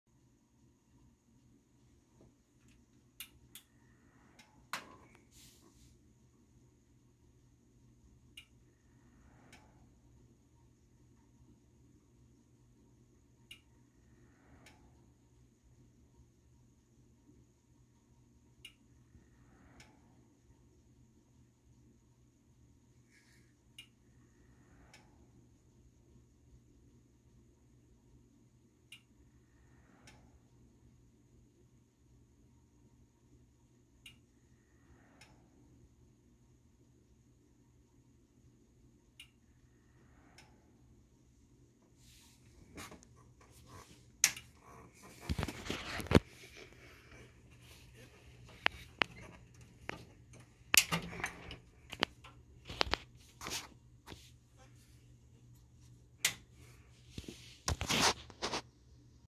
rip the storm killed it
There was a big storm and while I was streaming my pc got fried.